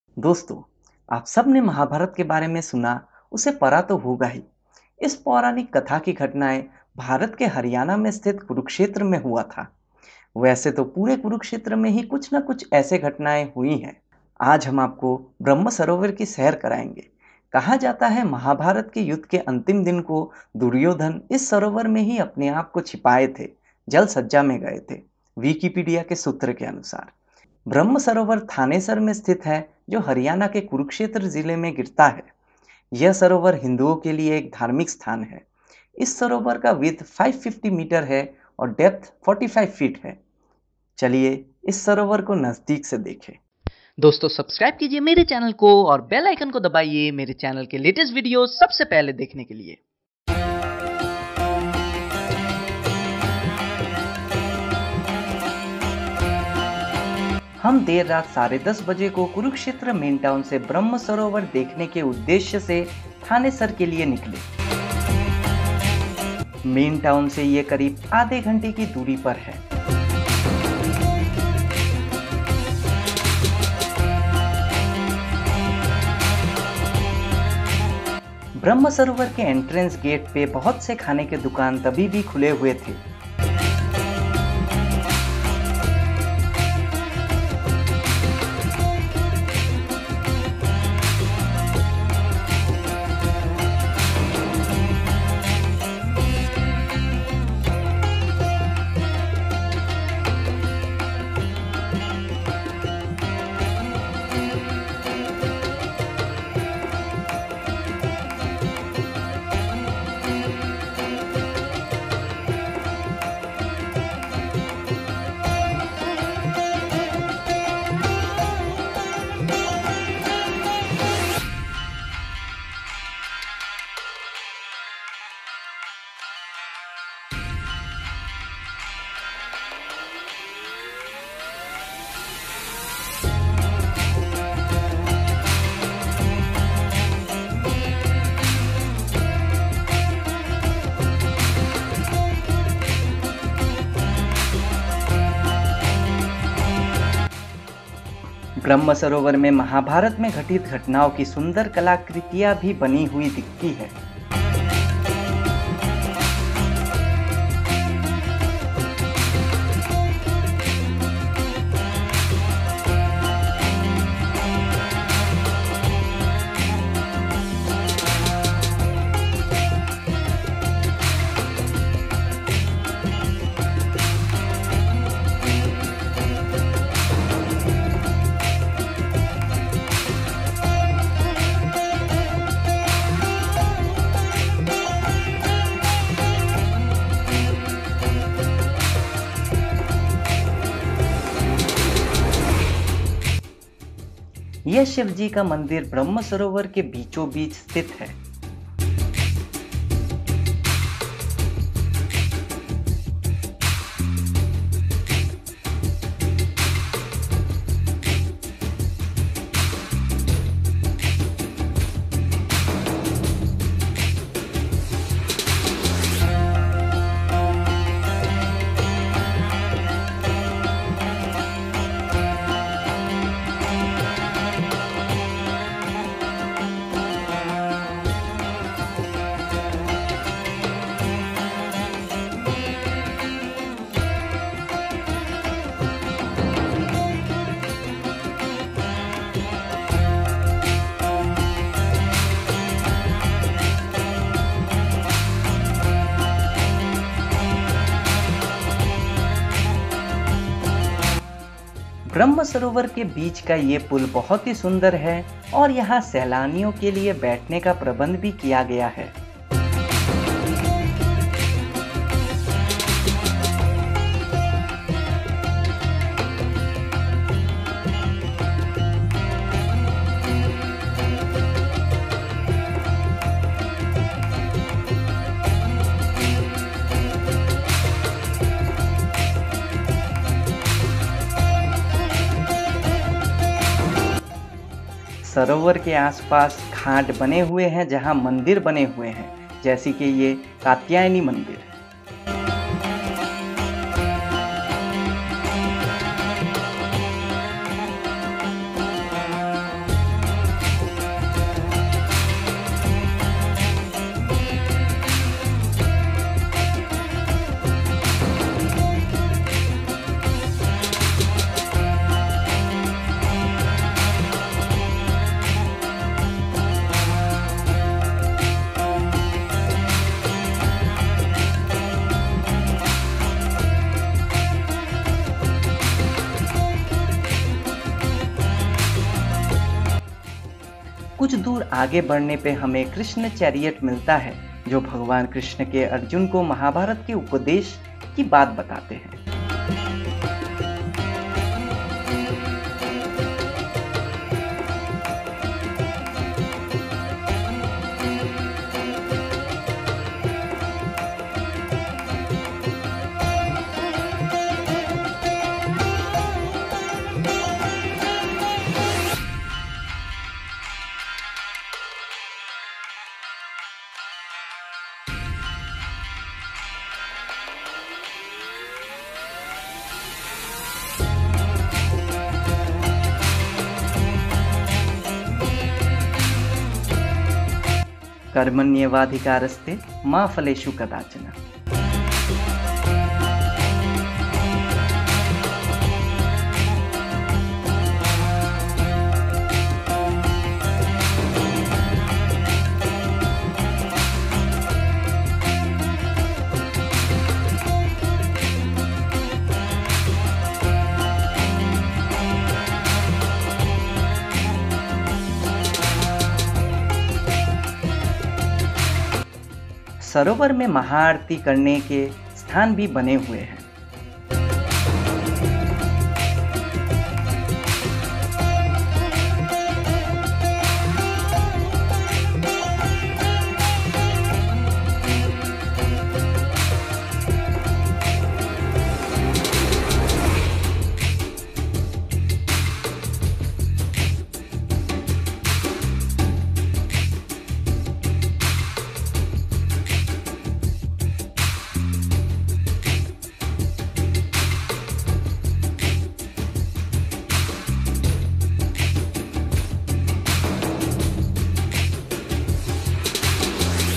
Brahma Sarovar Kurukshetra | Asia's Biggest Sarovar- Brahma Sarovar | Brahma Sarovar Video
Brahma Sarovar is an ancient water pool sacred to Hinduism in Thanesar, in the state of Haryana in North India. Hinduism lays emphasis on taking bath for internal and external purity. Most religious sites have water pools or Sarovar in or near the Hindu temple and Sikh gurdwara. The Hindu genealogy registers at Kurukshetra, Haryana are kept here.